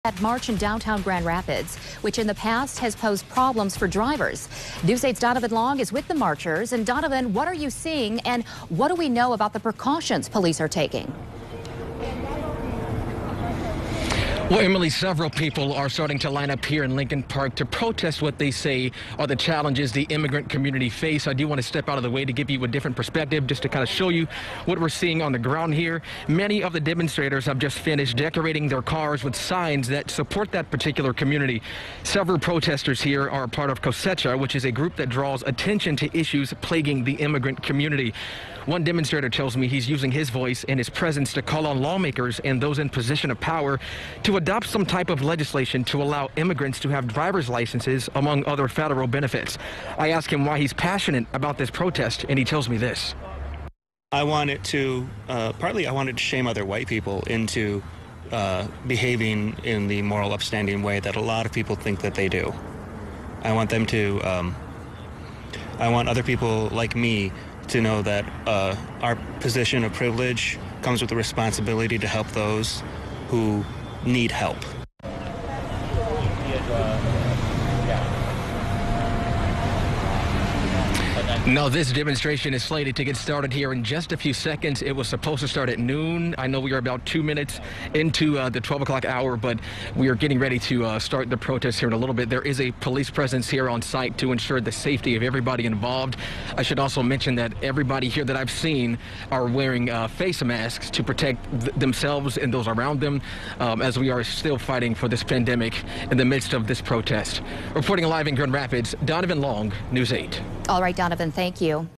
May Day protest in downtown Grand Rapids Friday
Dozens of activists gathered in Grand Rapids Friday to protest the challenges they say the immigrant community faces.
The protesters meet around 12 p.m. near Lincoln Park on Bridge Street NW in the John Ball area. Many of the demonstrators decorated their cars with signs to take part in the vehicle parade.
Several of the protesters are part of Movimiento Cosecha, which is a group that draws attention to issues facing the immigrant community.

One demonstrator said he is using his voice and presence to call on lawmakers and those in position of power to adopt some type of legislation to allow immigrants to have drivers’ licenses among other government benefits.
“I wanted to shame other white people into behaving in the moral upstanding way that a lot of people think that they do,” said Mike LaRowe. “I want other people like me need to know that our position of privilege comes with a responsibility to help those who need help.”

Officers with the Grand Rapids Police Department were present at the event.
On Thursday, GRPD said officers would work to ensure people can peacefully express their First Amendment rights in a public forum while maintaining safety and access to essential services like hospitals.
The department also reminded people who do not need to leave their homes for work or essential needs should follow Gov. Gretchen Whitmer’s stay-at-home order.